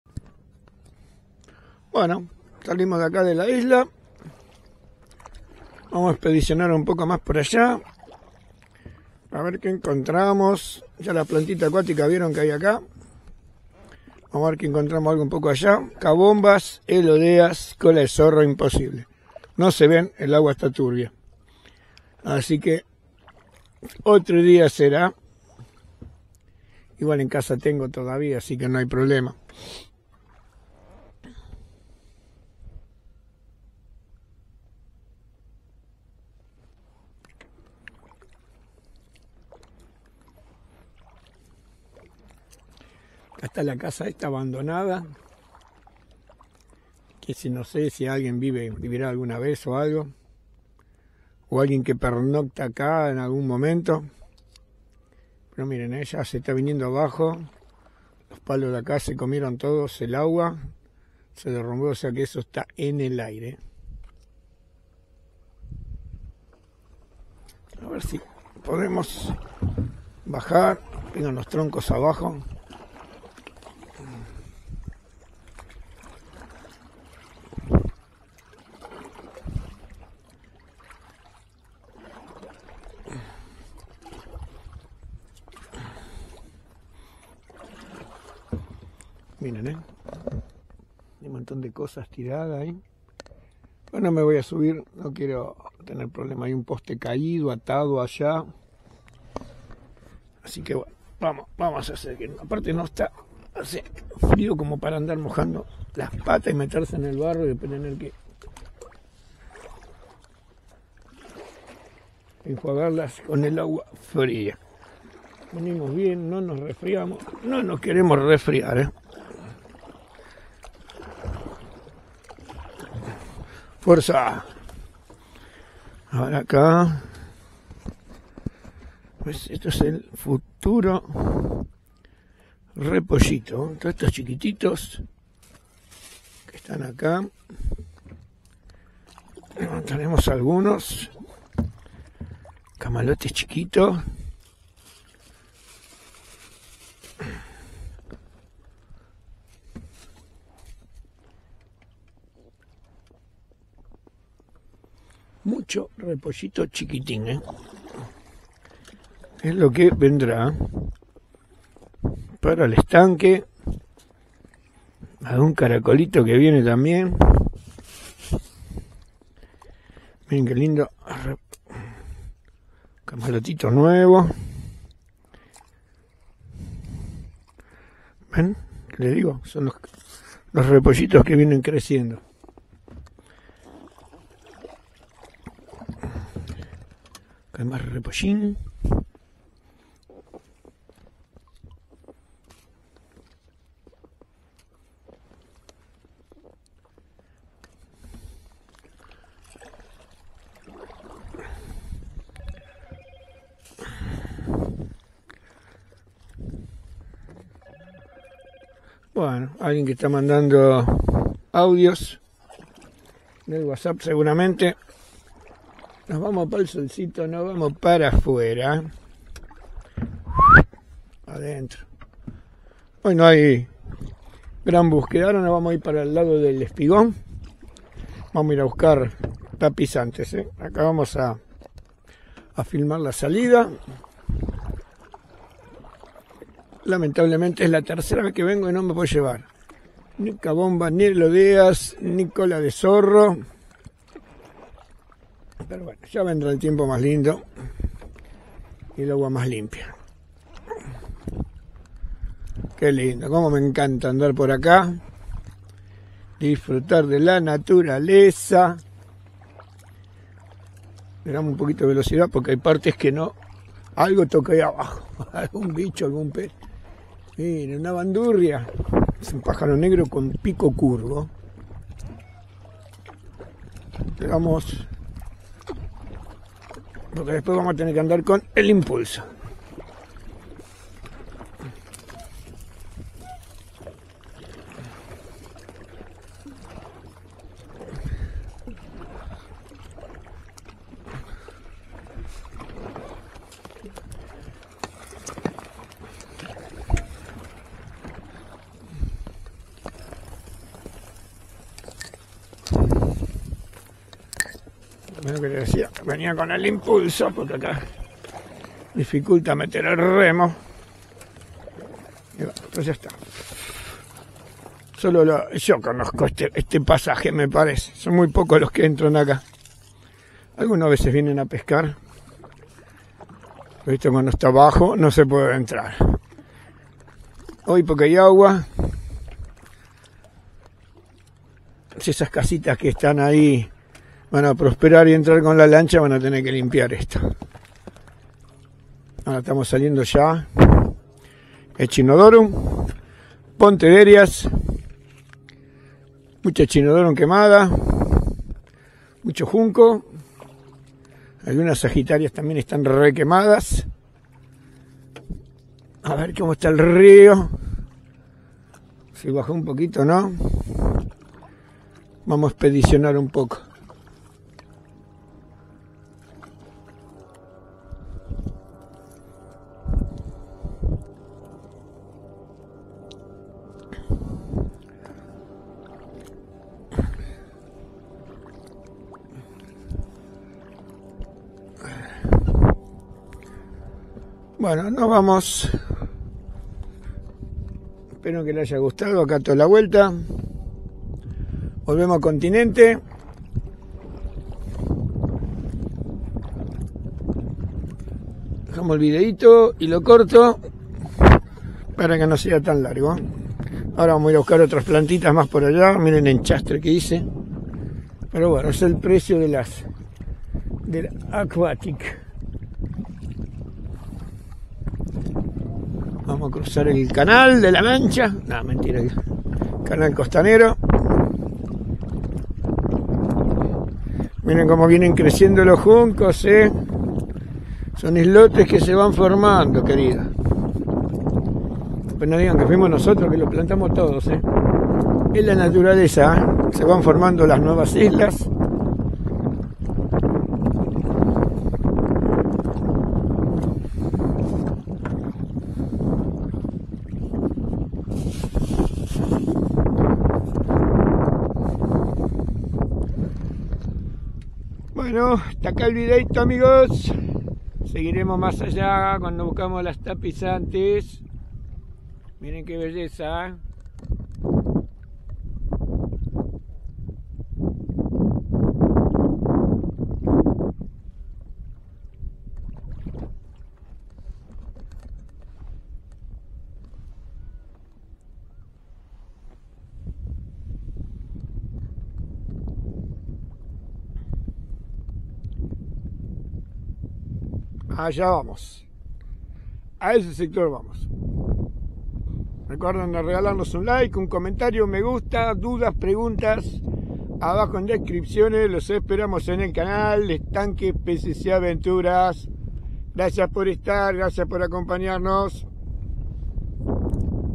explorando y buscando acuaticas y palustres para estanques y peceras
hola hola compas nuevo video en pleno invierno aprovechando el poquito sol y tregua con los gelidos dias salimos a remar por el rio de la plata y entrando al delta en la seccion mas nueva